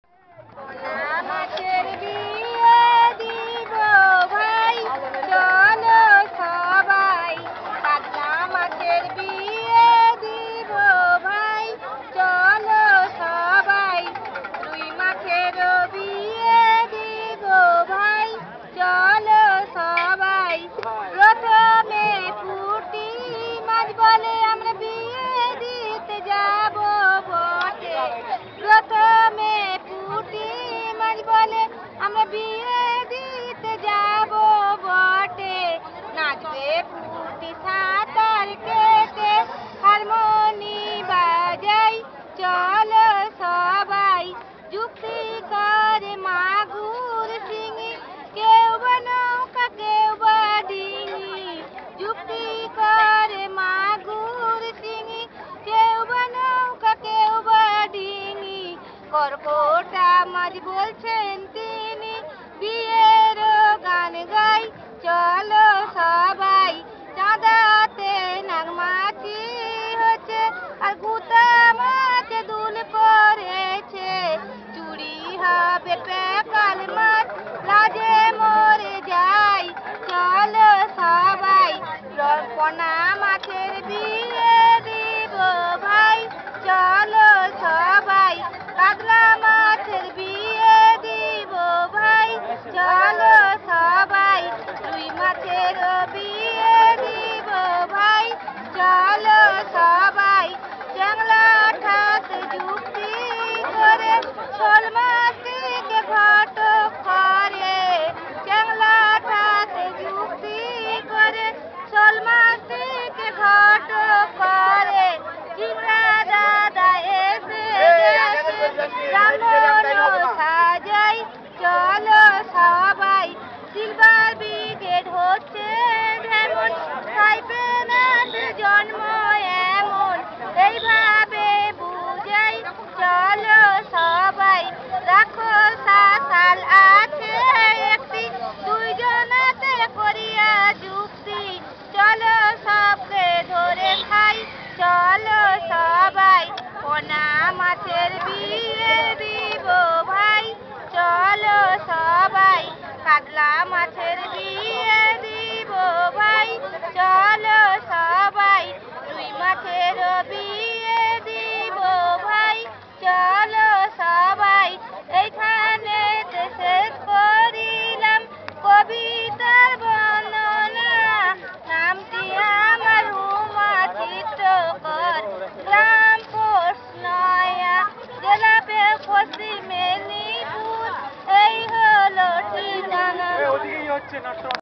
Pot er Gan(one of rear folk culture or song or entertainment of rural Bengal)
once up on a time no radio, no tv, no cinema in rural Bengal, these time this type of artist fast drawing so many subject about social life, history. and run village to another village & show this to villager's with song for entertainment, after look this villager's also given  some money & food, but now this type culture is slowly slowly finished for present electronic entertainment. this drawing now only show for decoration in the drawing room of richest people.